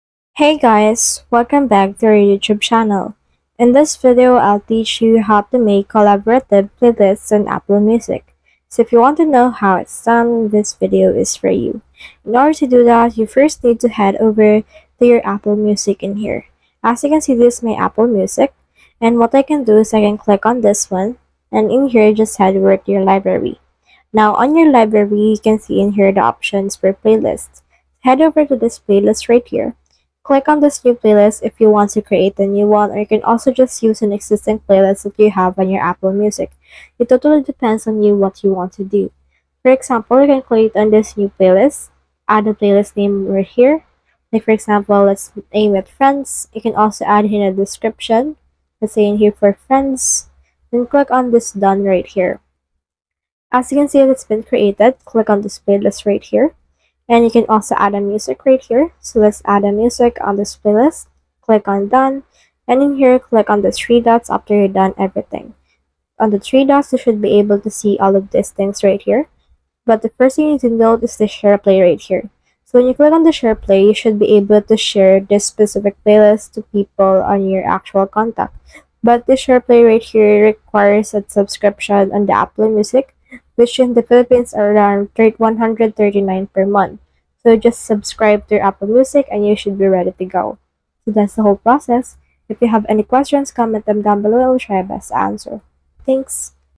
How to Make Collaborative Playlist on Apple Music
How to make a collaborative playlist on Apple Music? In this tutorial, I show you how to create and share a collaborative playlist on Apple Music. This means you can let other people add or remove songs in your playlist on Apple Music. Apple Music collaborative playlist are great to share with friends or family.